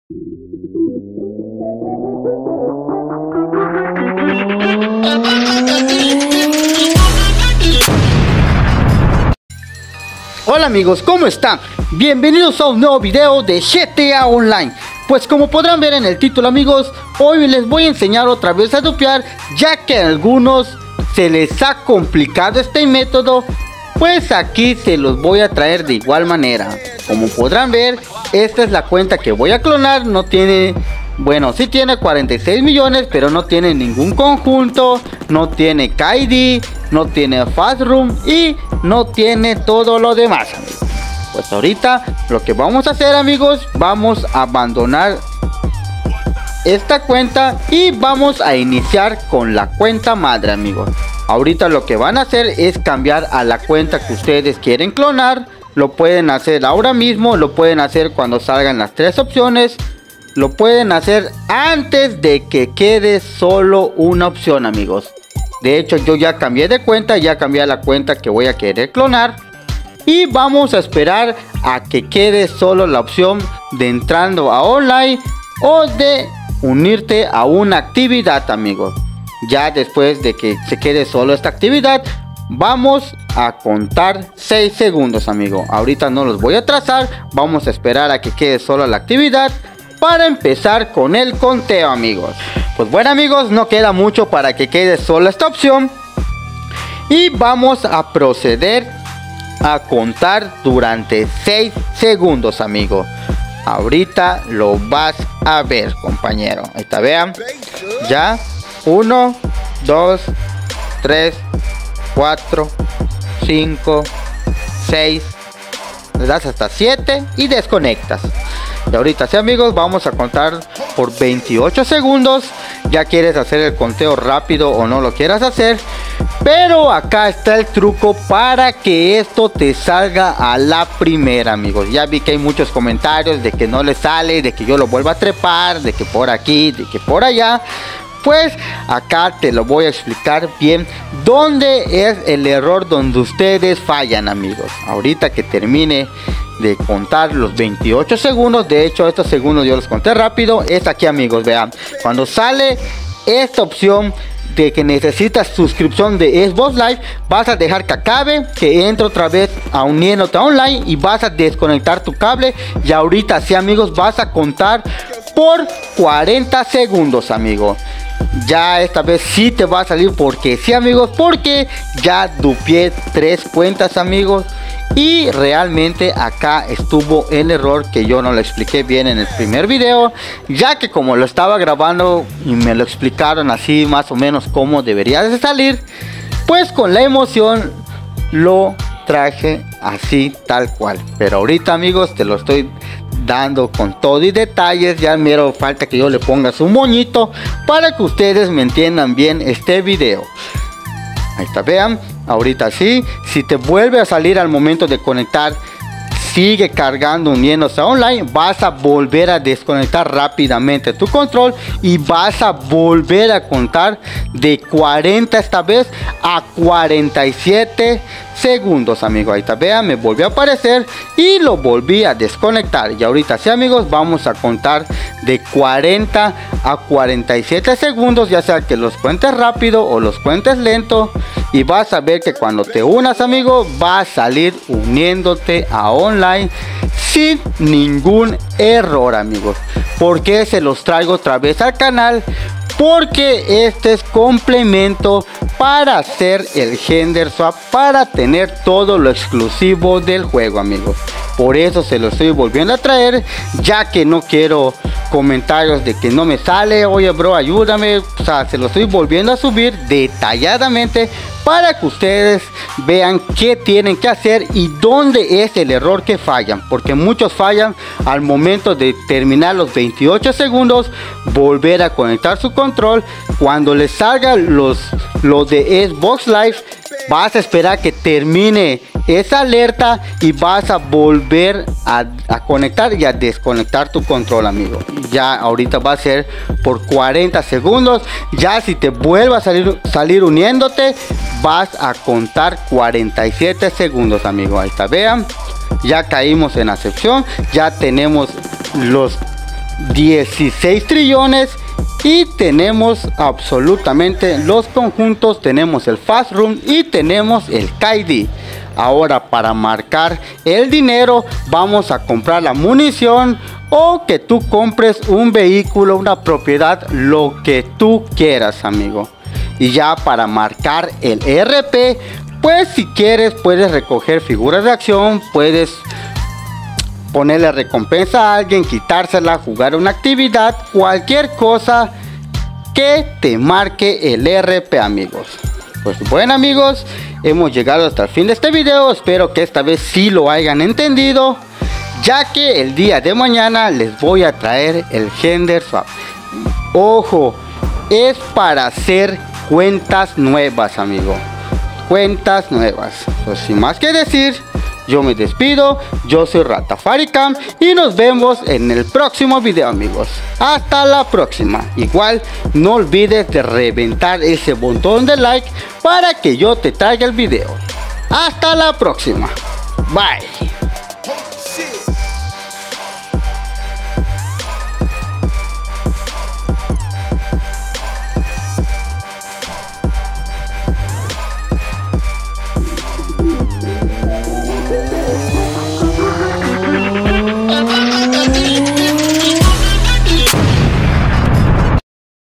💥NEW VIDEO COMO DUPPEAR O CLONAR TU CUENTA MEJORADO Y MEJOR EXPLICADO SÓLO FACIL APRENDELO CRACKS😎
HOLA AMIGOS AQUI LES DEJO OTRA VEZ EL METODO DE DUPPEAR YA QUE NECESITO QUE SE LO APRENDAN BIEN PARA QUE PUEDAN OCUPAR EL SIGUIENTE TRUCOS AMIGOS ESPERO SUS LIKES COMENTARIOS YA QUE ES NECESARIO ESTE TRUCO SALUDOS

xboxseriesx xboxone ps4 ps5 #modearcoches